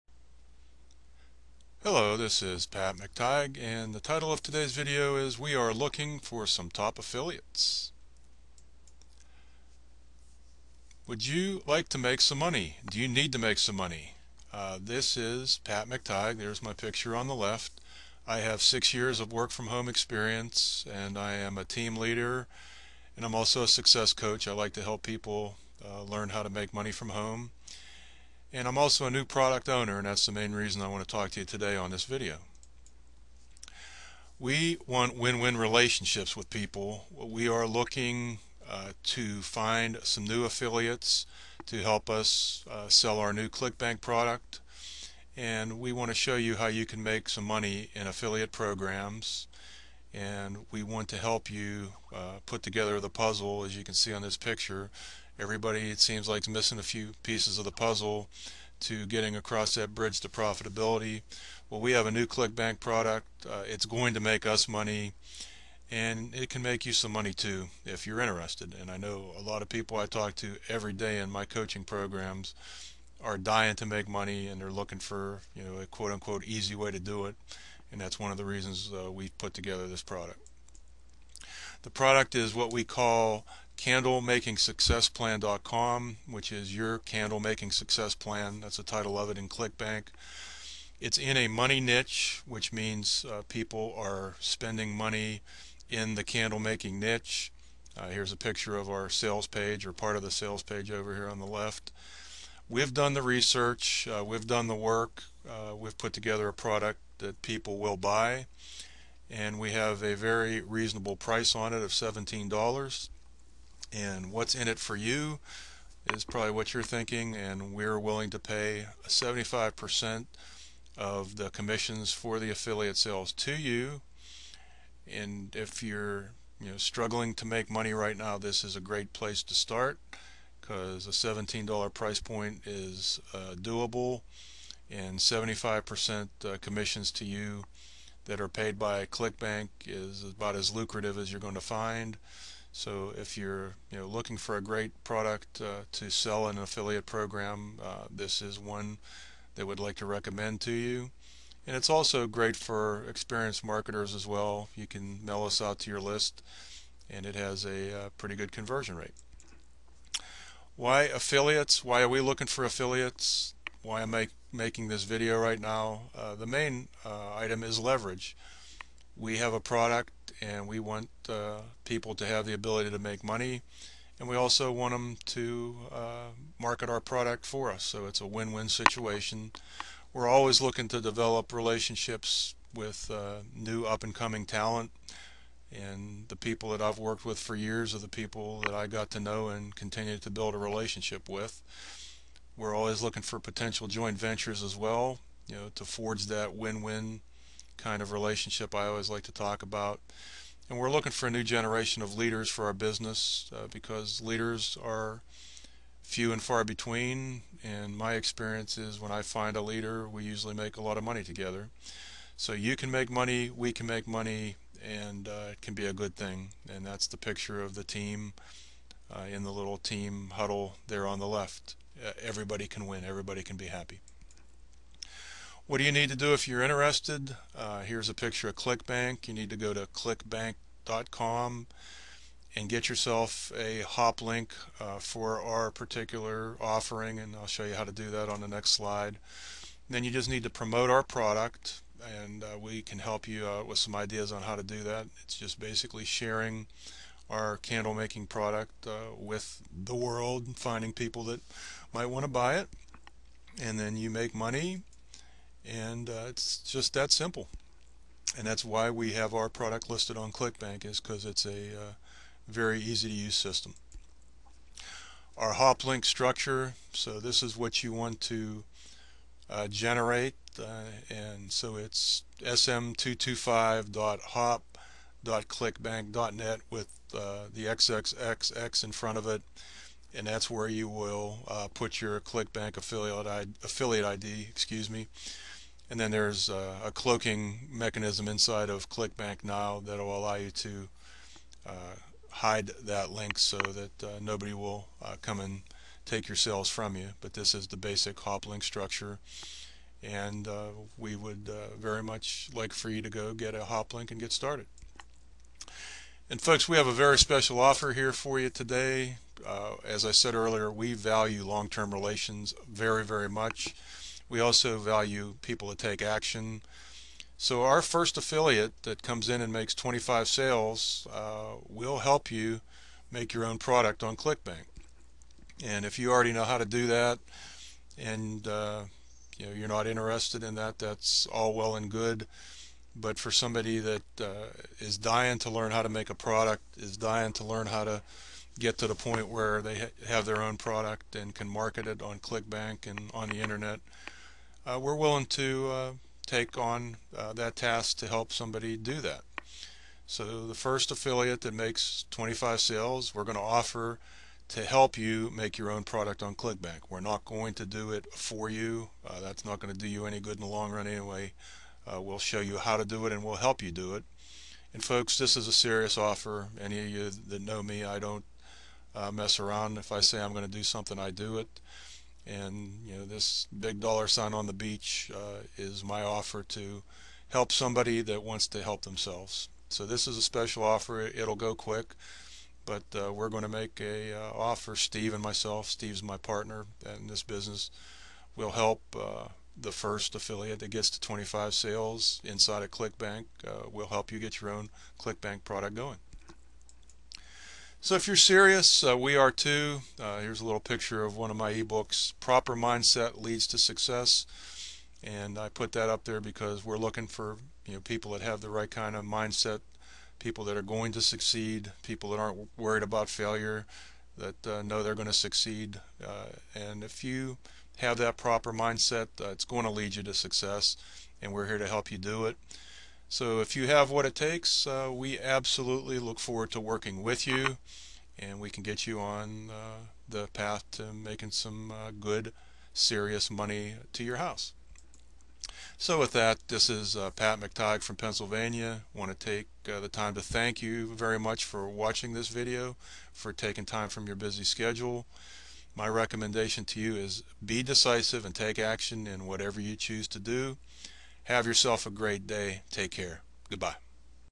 Looking For Candle Affiliates
Patrick McTigue explains his affiliate program for his new Clickbank product.  Pat covers the compensation plan and a special offer for a top affiliate.